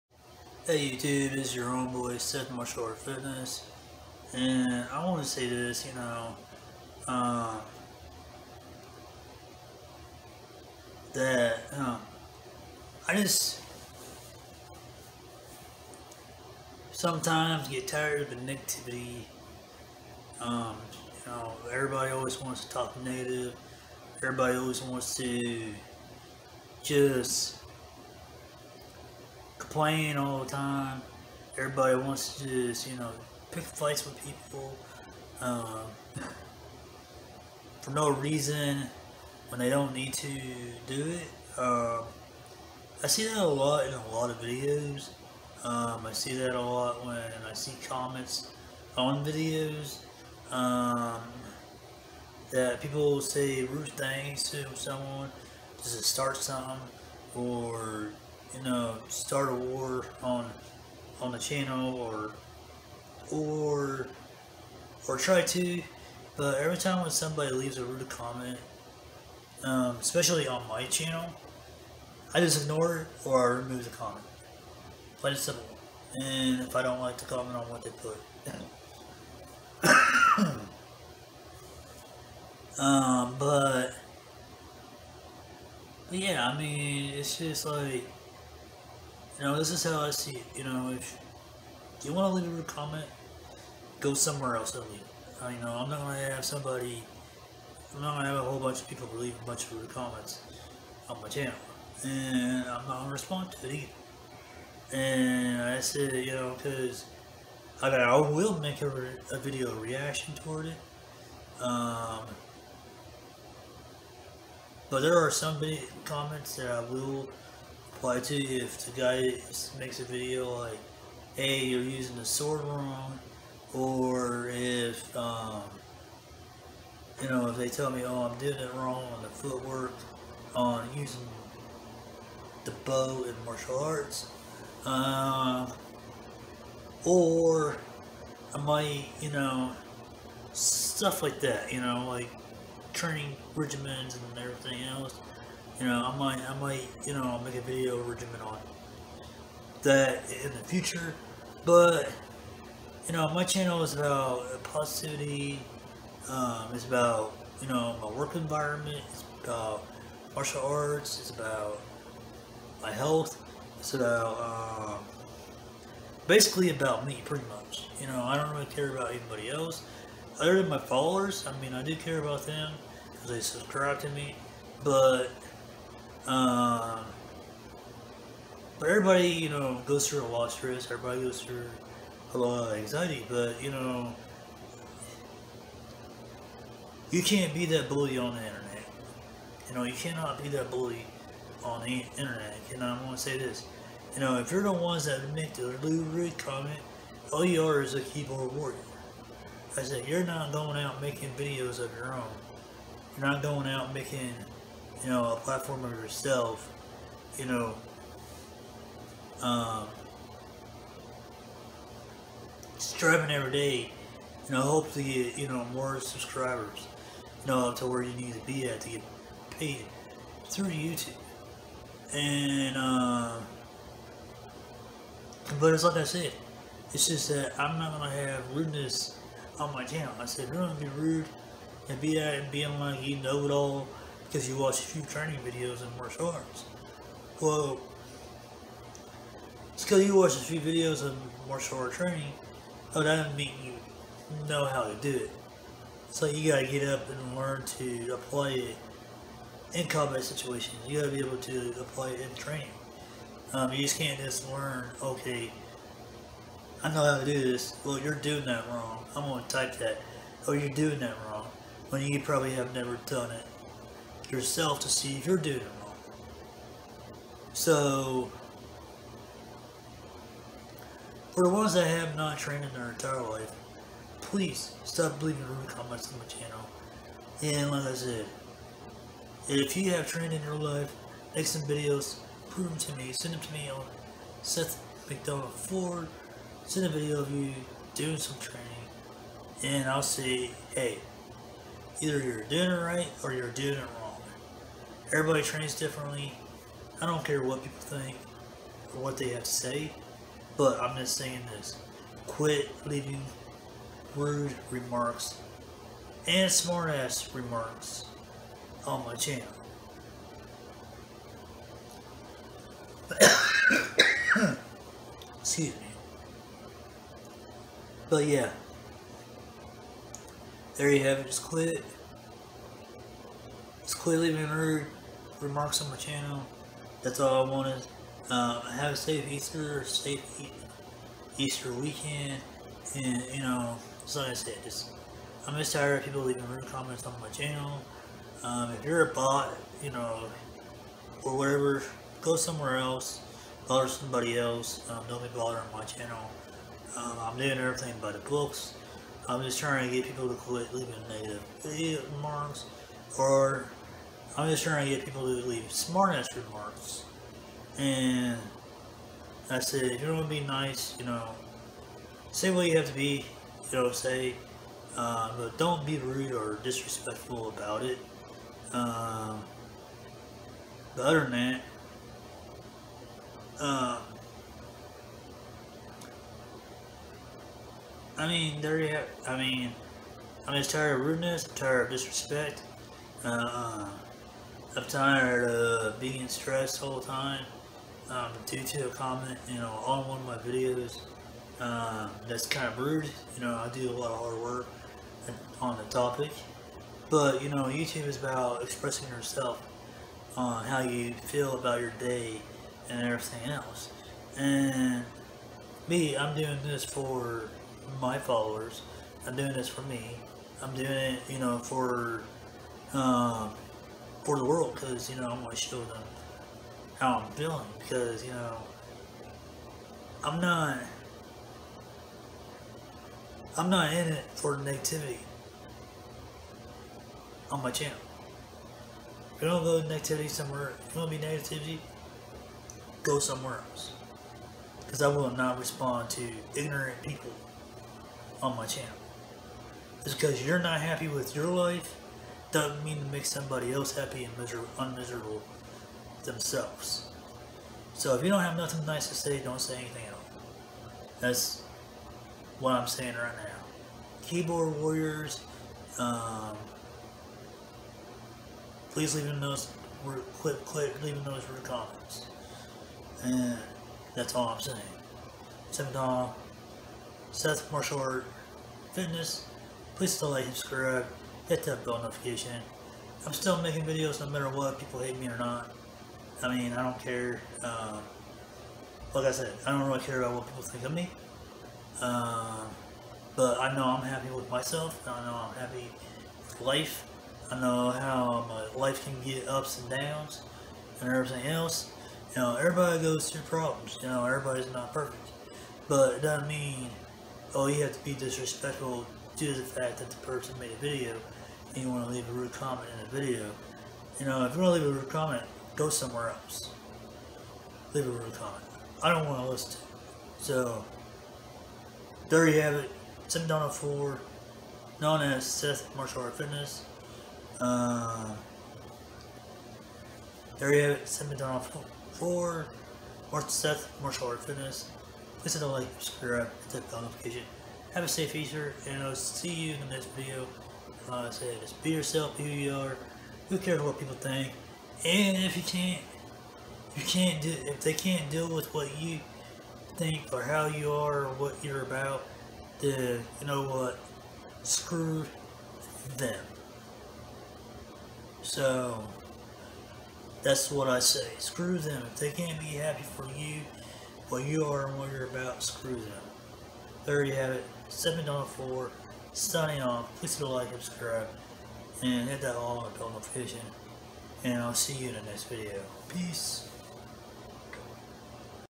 Quit Leaving Rude Comments
Quit leaving rude comments on my videos if your not going to be respectful I am going to just start ignoring them on the remarks that are negative but if they are positive I respond but if they are being a smart ass remark or rude like in a way I either one ignore them or just remove them from the comments if I have to my channel is bout positive and it's bout me only no one else.